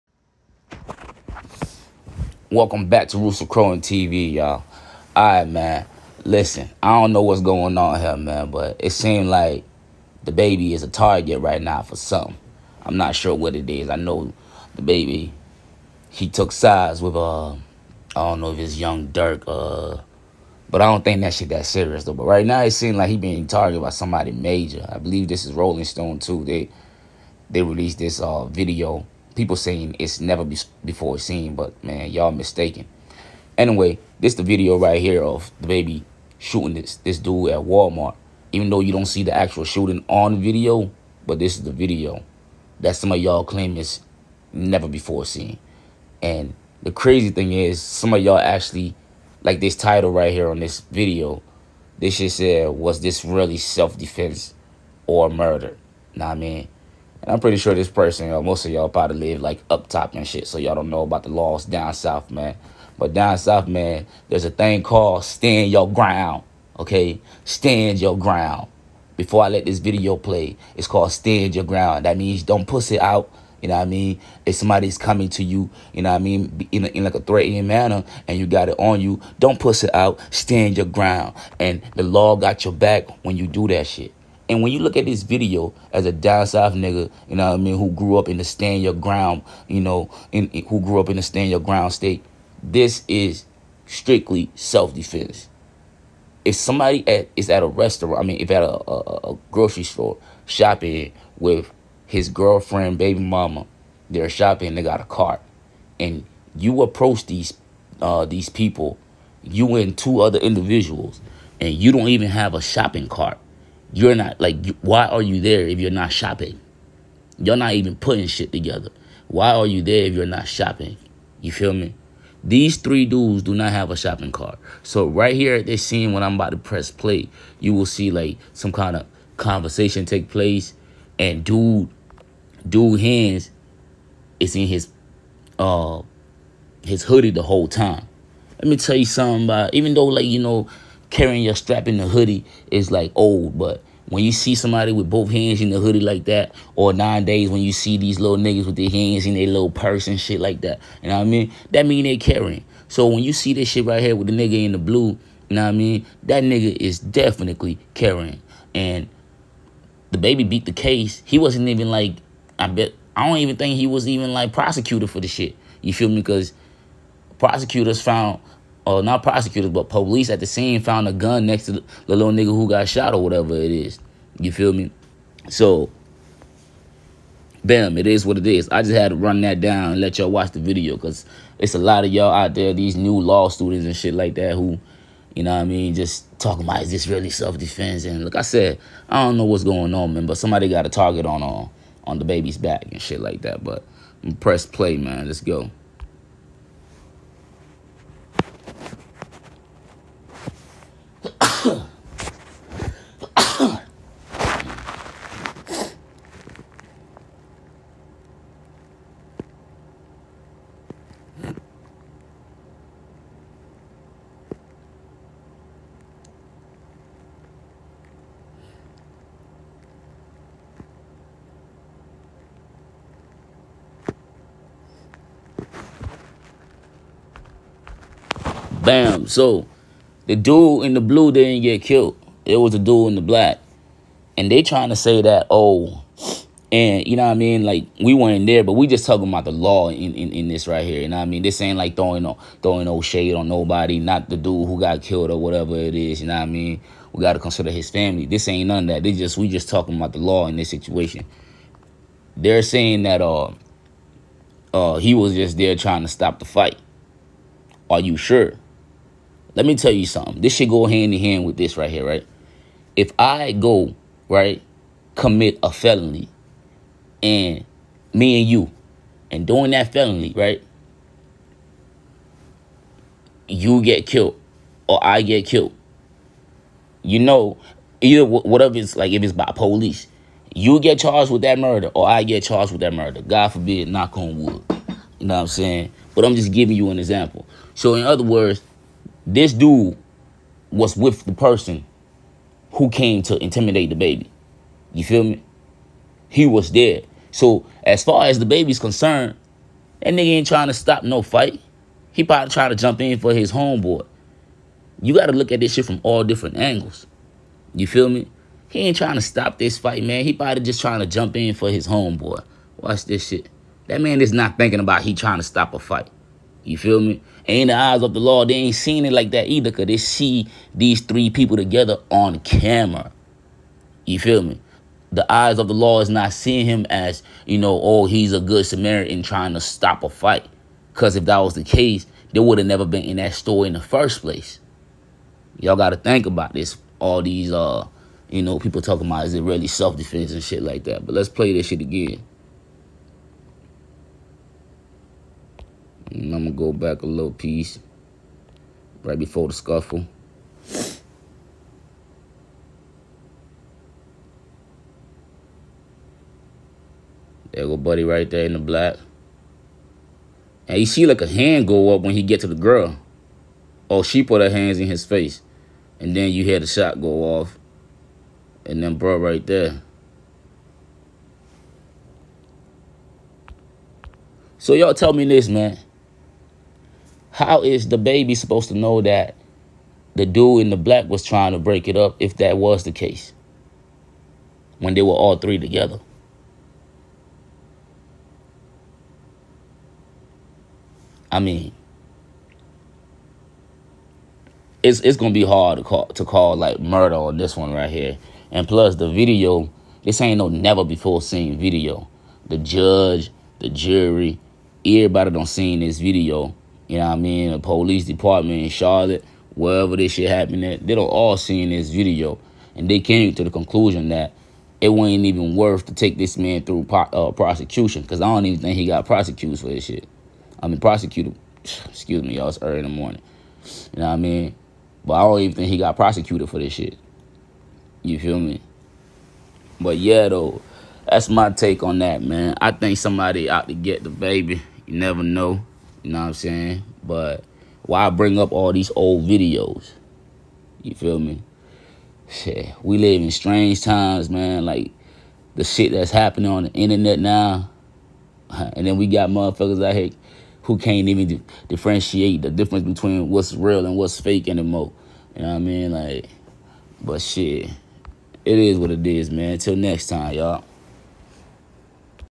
Dababy newly released video of the Walmart incident
Who release this video now and why? Some people are wondering what is murder or self-defense?￼